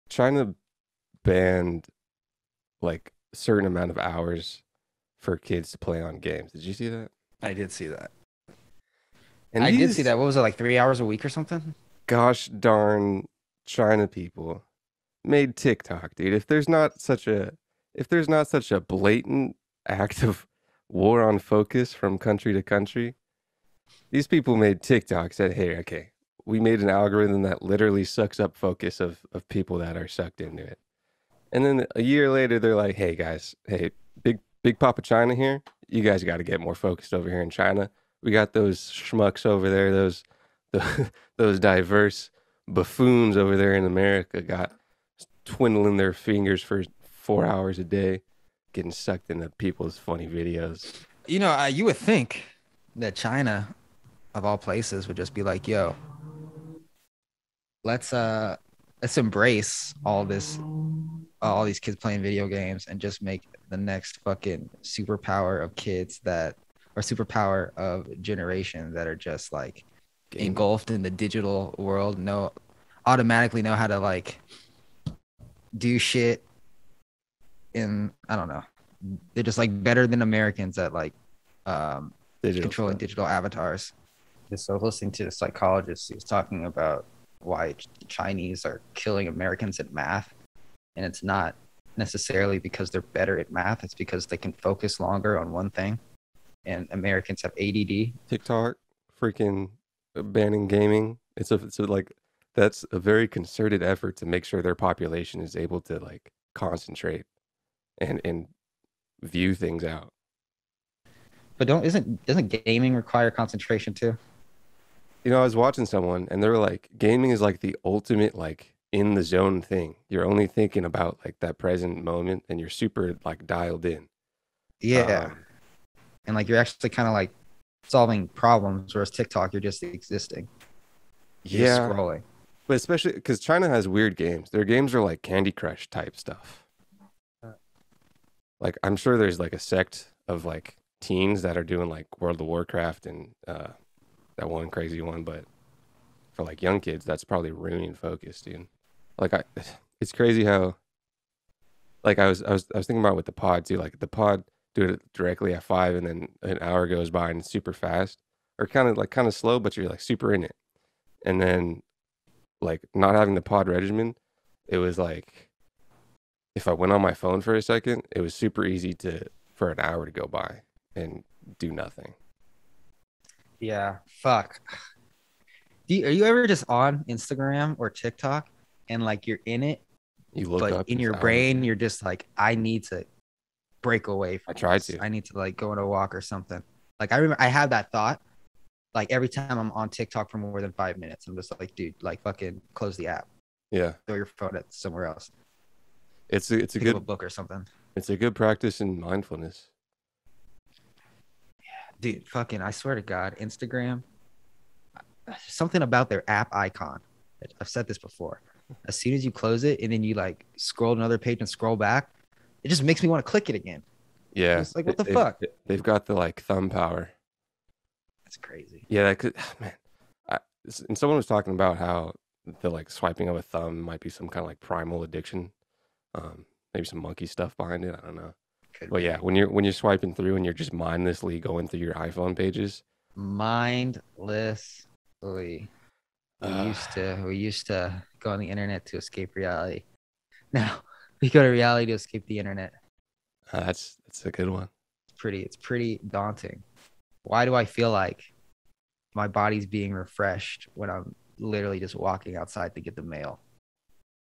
Why China Is Better Than Us
Will kids in China have better attention and retention now that their government forced them to get off their phones?

The authoritarian methods are probably not ideal, but the parental nature of the order is seemingly valid.

What do you think? Is China positioning itself to be better than the world?